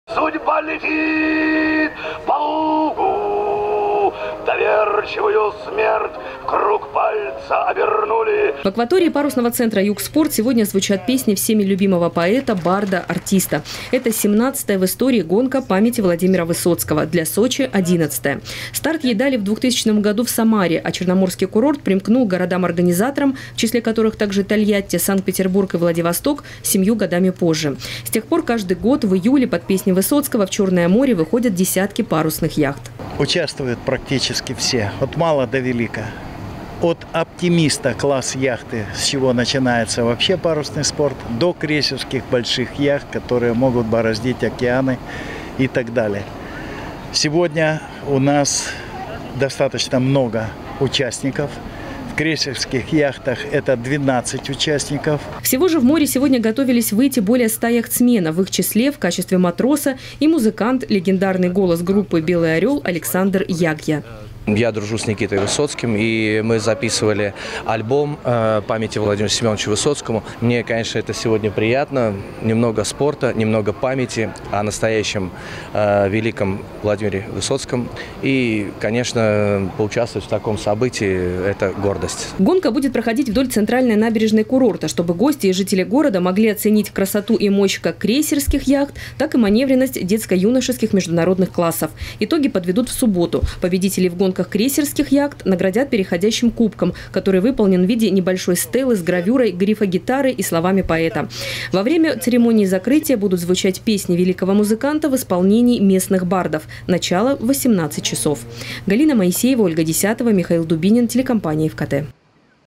В Сочи стартовала парусная регата памяти Владимира Высоцкого. Новости Эфкате
Это 17-ая в истории гонка памяти Владимира Высоцкого, для Сочи — 11-ая.

Старт ей дали в 2000 году в Самаре, а черноморский курорт примкнул к городам-организаторам, в числе которых также Тольятти, Санкт-Петербург и Владивосток, семью годами позже. С тех пор каждый год, в июле под песни Высоцкого в Чёрное море выходят десятки парусных яхт.